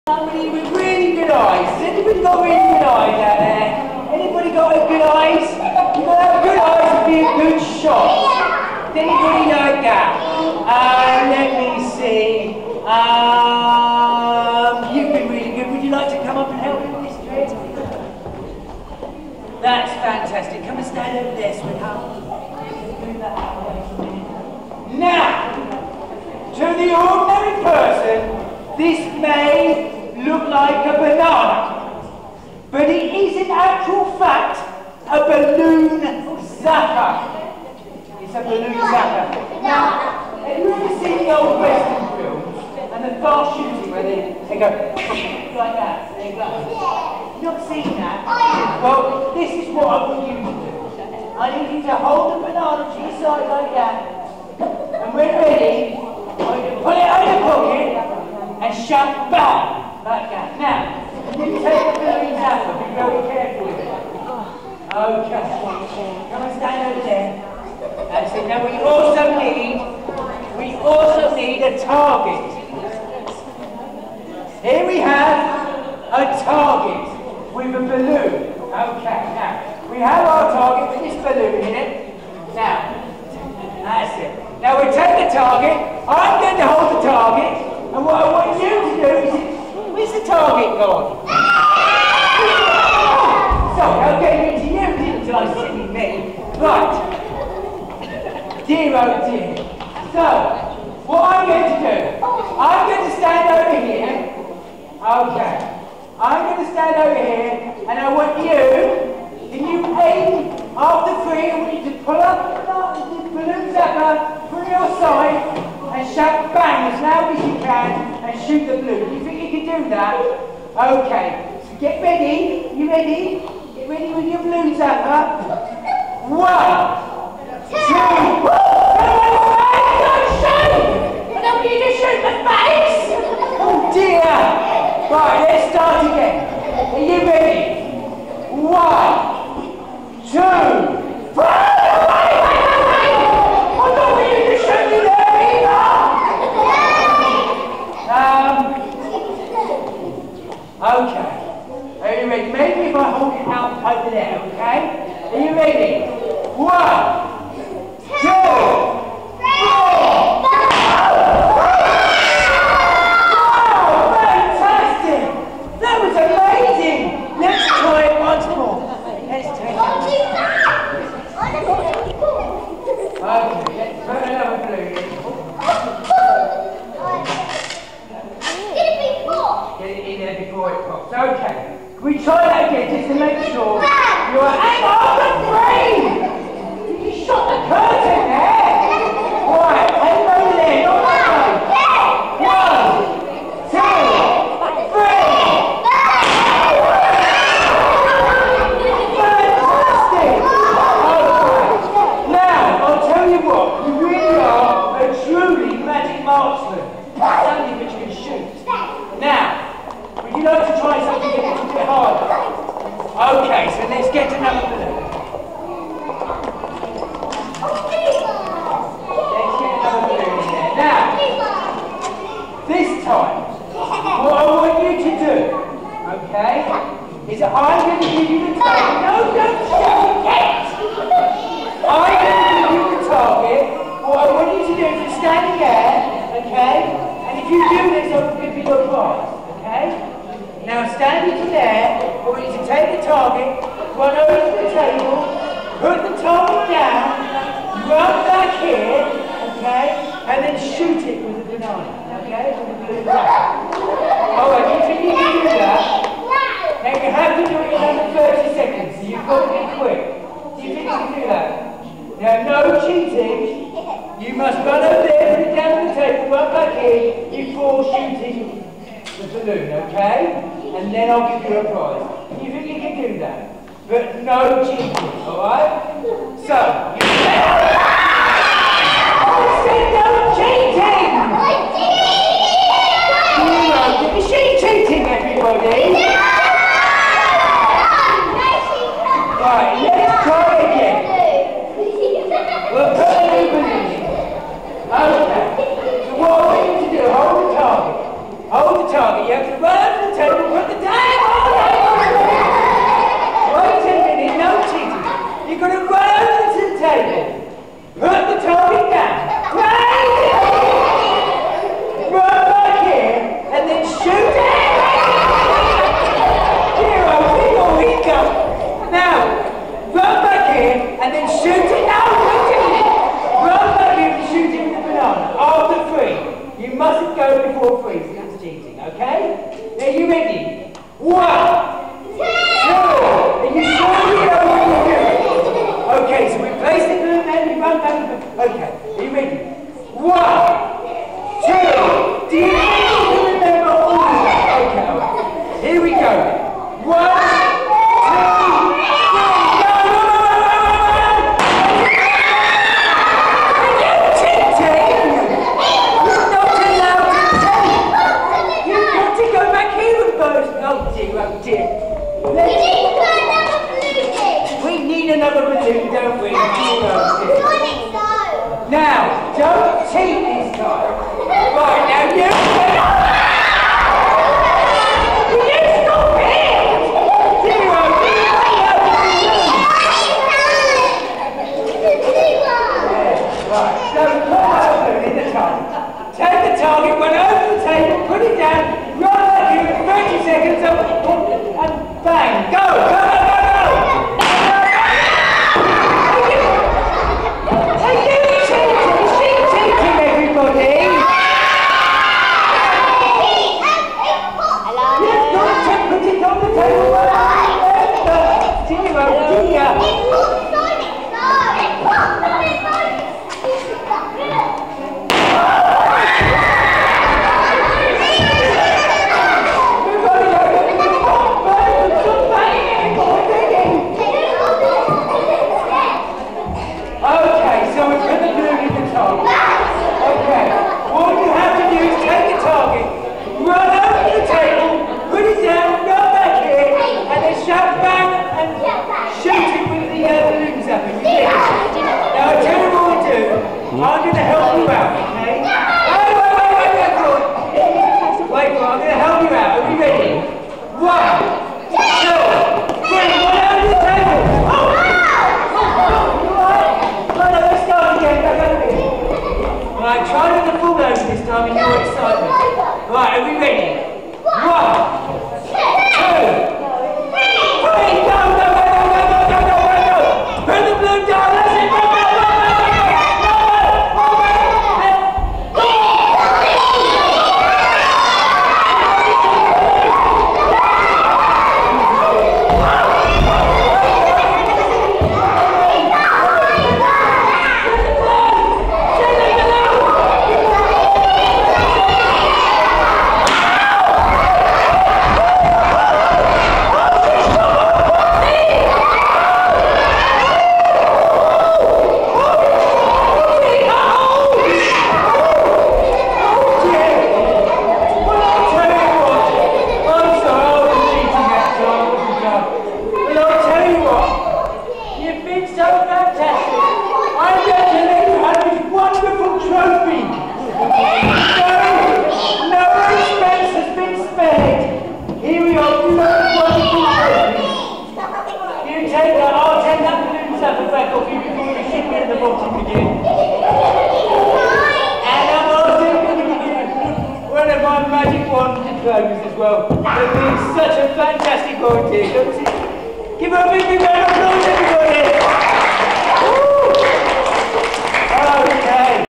Magic Marksman
Using a balloon zapper (or a banana to the untrained eye) the magician asks his young assistant to shot a balloon in a target, after a few successful tries the magician asks his helper to complete one last challenge for a prize. What follows is mad comedy chaos as the assistant tries to complete the final task!